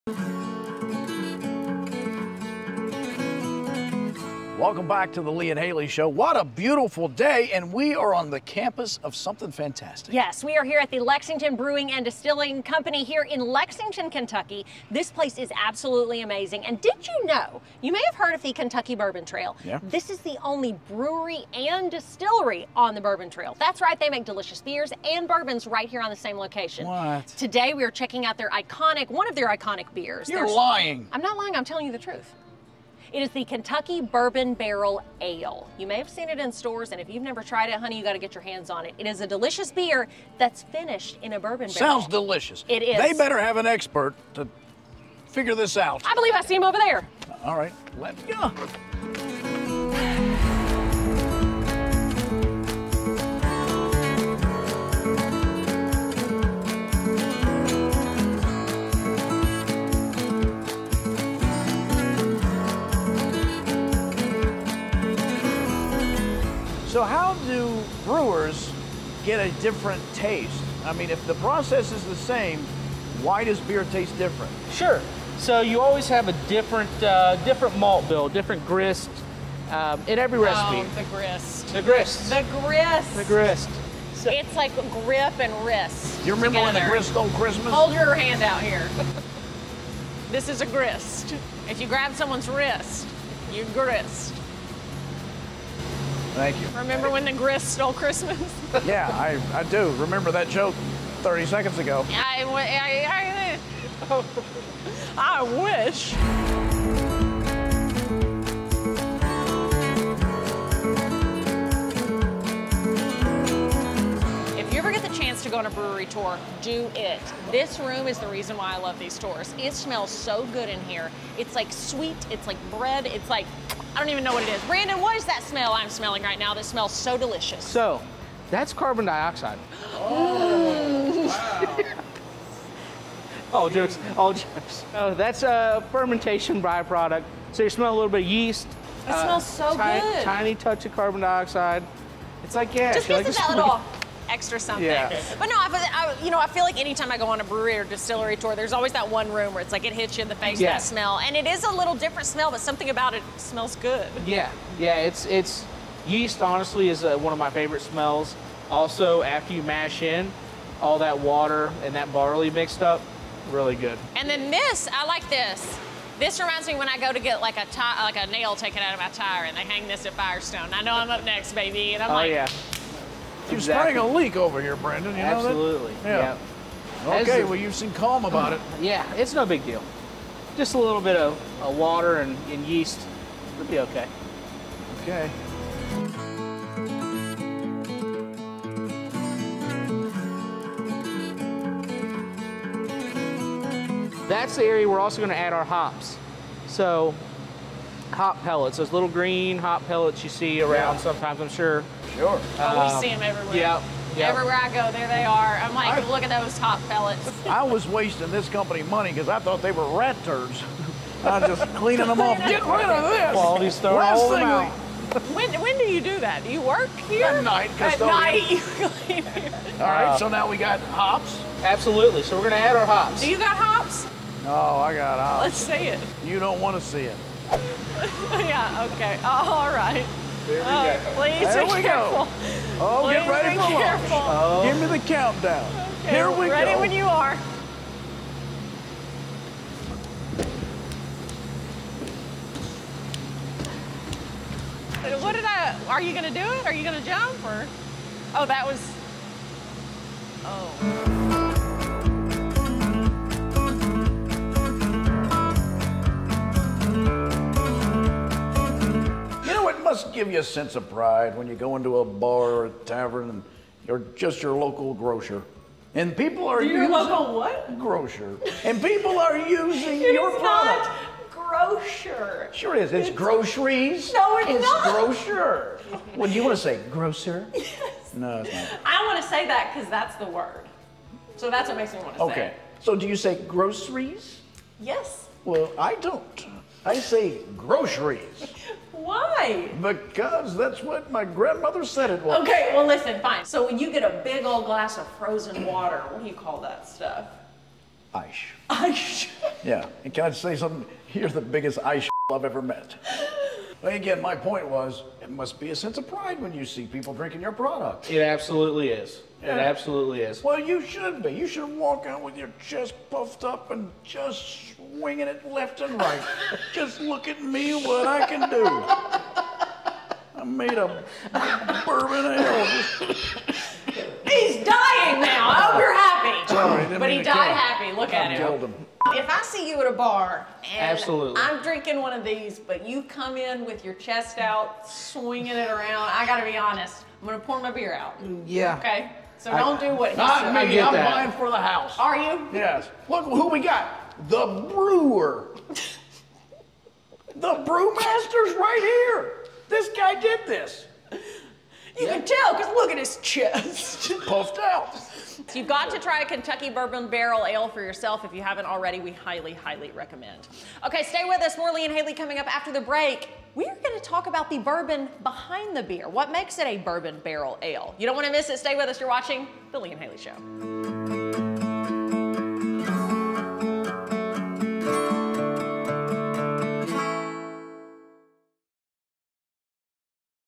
Lexington Brewing And Distilling Company Part 1
Lee and Hayley go on a tour of the Lexington Brewing & Distilling Co.!